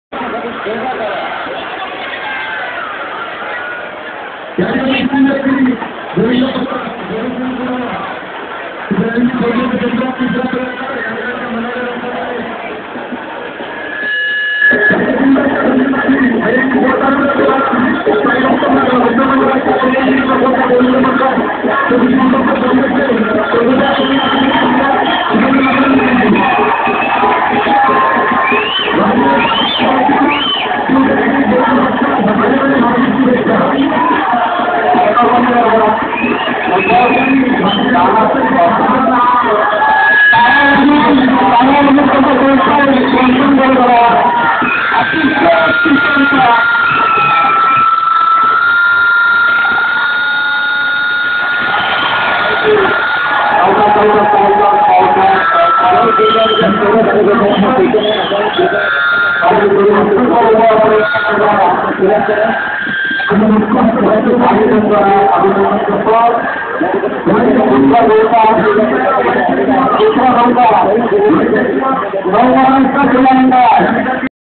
Horrifying 25 ft fall while attempting 7 tier dahi handi
Govinda Mandal from Shiv tekdi attempting second time to form a 7 tier pyramid and fails. Kid falls from approximately 25 ft height but is safely caught by people below. Great stuff!